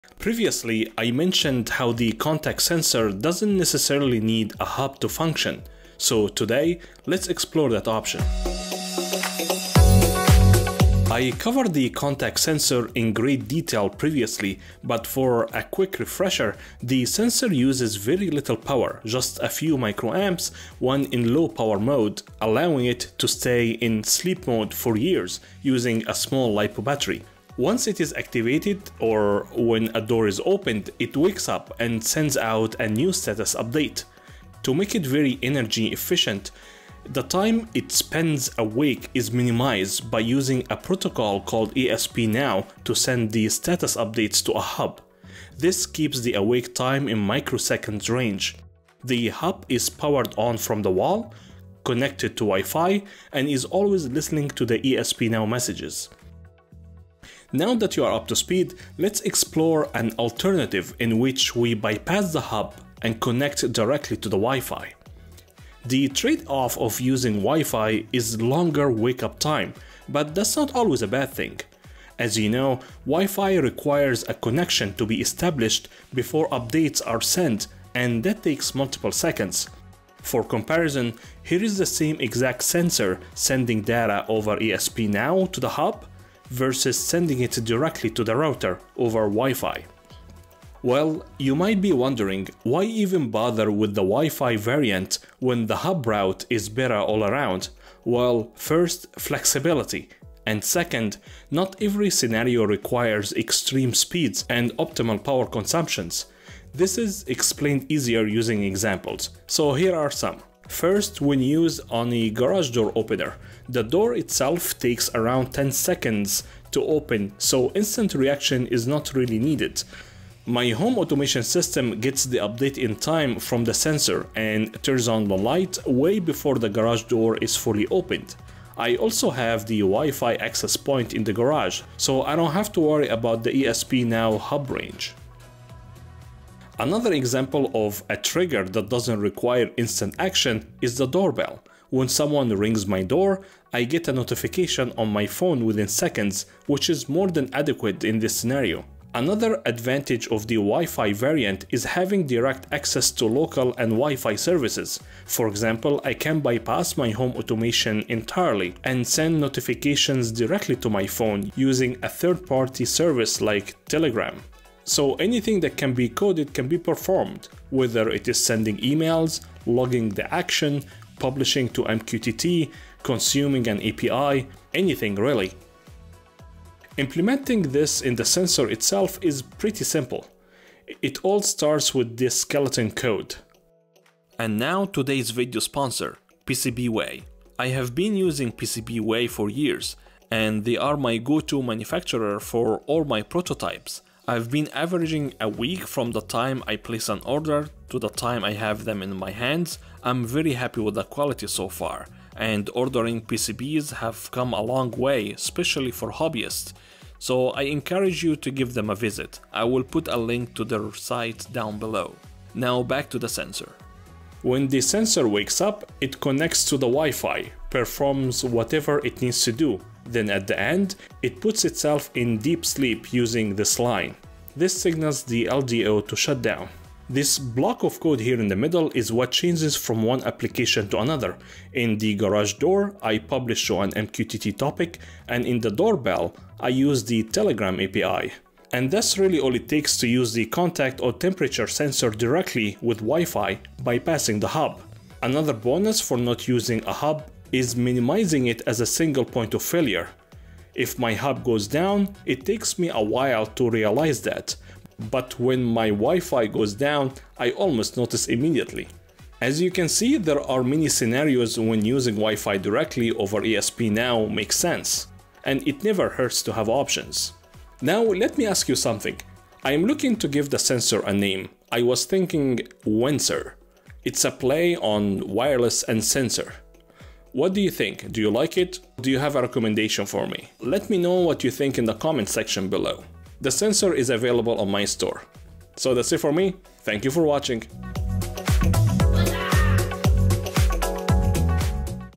Why Did I Pick Wi-Fi over ESPNow?
An overview video of version 3 of my contact sensor.

My list of ESP8266 Dev boards

00:00 Intro
00:10 refresher
00:57 Going Hubless
02:57 Sample code
03:04 sponsor
03:34 Sample code
04:04 Bonus
04:27 Picking a name
04:43 Outro